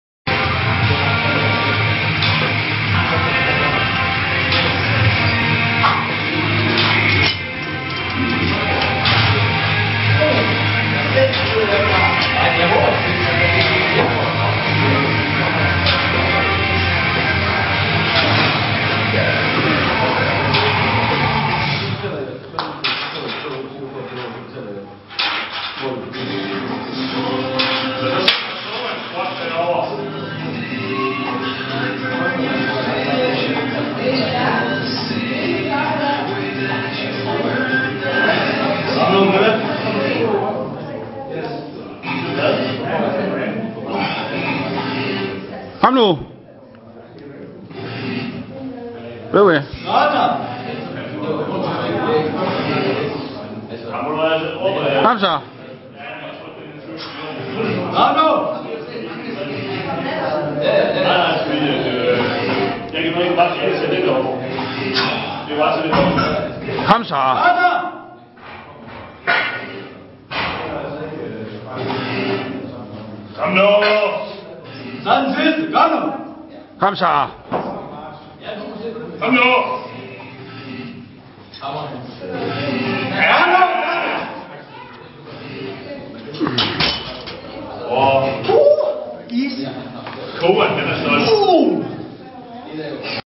Squat 10 x 165kg
Oct. 30th 2009, 10 reps @ 165kg(364lbs) weighing in at 92.4kg(204lbs) in training clothes.